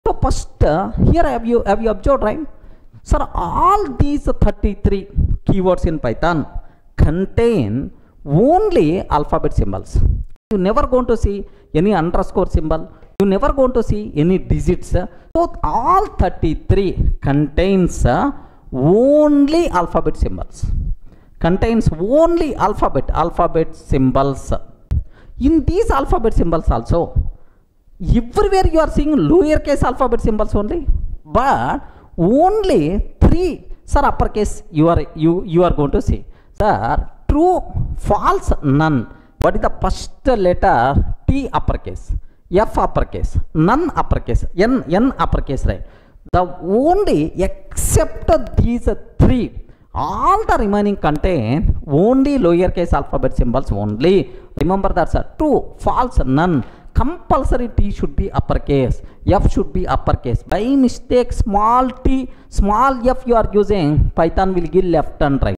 Python Keywords: Mastering the 33 Reserved Words
Python keywords primarily use alphabet symbols, with no underscores or digits. Lowercase letters dominate, except for 'True,' 'False,' and 'None,' which start with uppercase letters. These three are the exceptions to the lowercase rule.